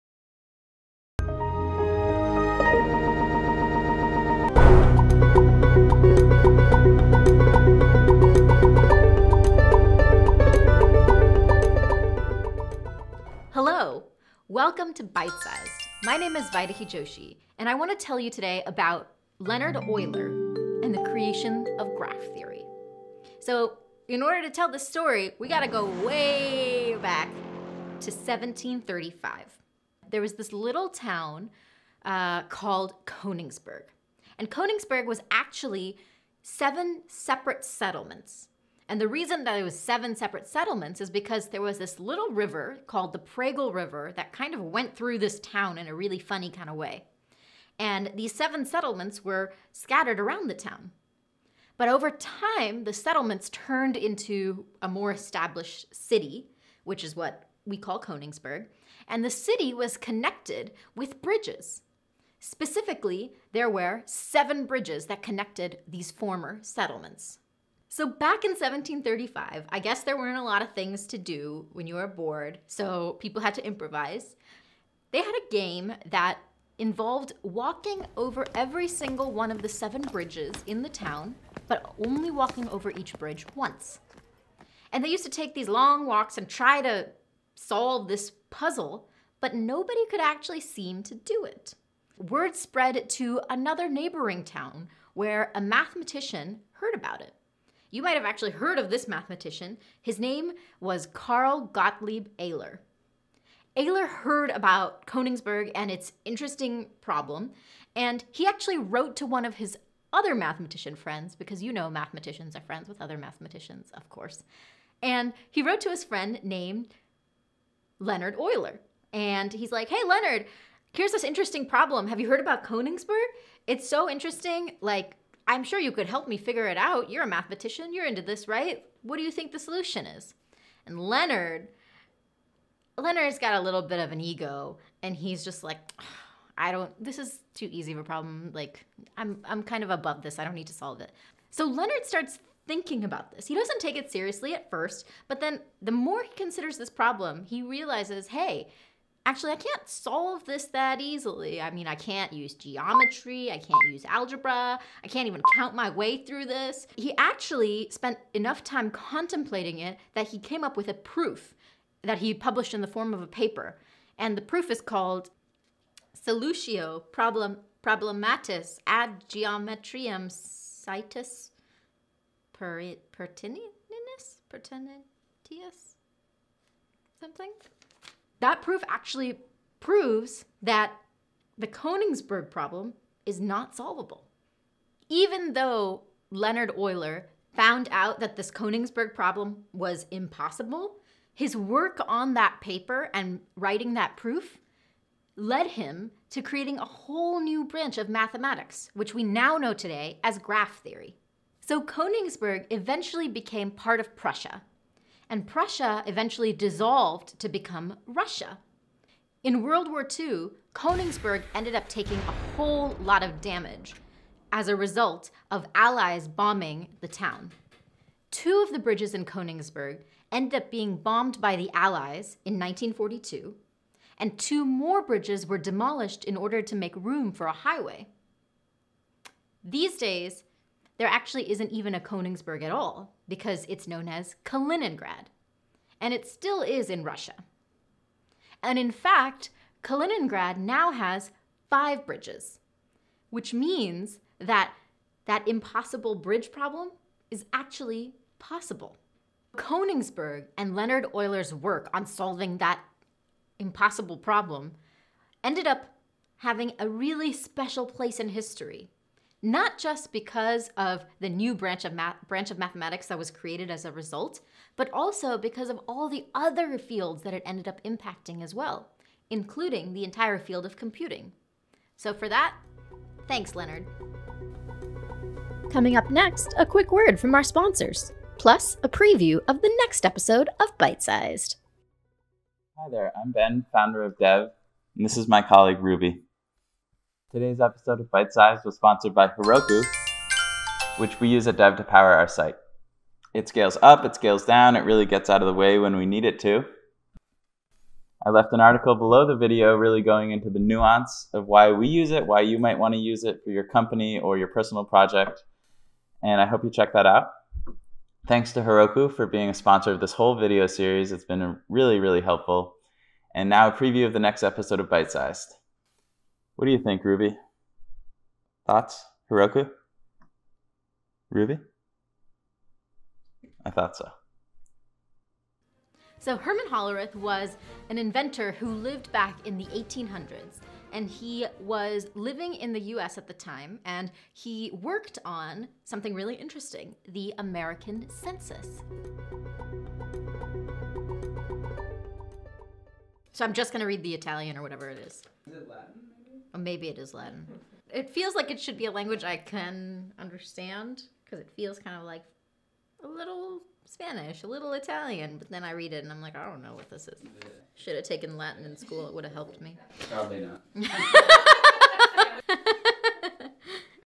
Byte Sized Episode 2: The Creation of Graph Theory
Today's episode of Byte Sized is about Leonhard Euler and the creation of Graph Theory.